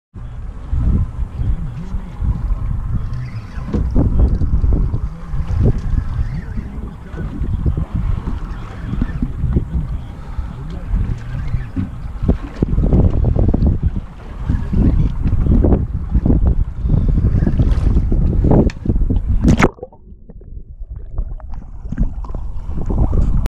Atlantic Beach Big Fish Charter_Salt Air Ventures_Fishing
BOB & ERIC's full day of offshore fishing, battling Amberjacks, triggerfish, black sea bass, B-liners and a bunch more.   Book your trip with Salt Air Ventures